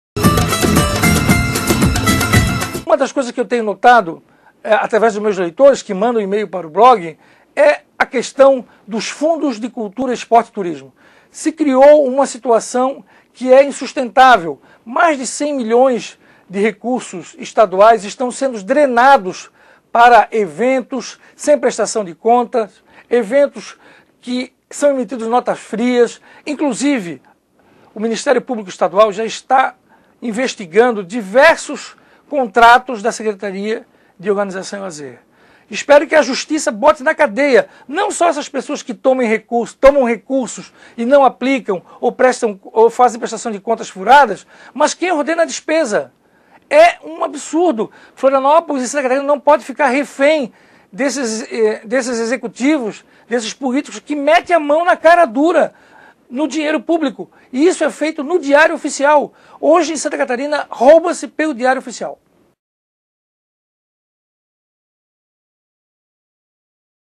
TV Mosquito - Santa Catarina: Rouba-se pelo Diário Oficial
Blog Tijoladas do Mosquito informa sobre a corrupção em Santa Catarina através dos Fundos Seitec.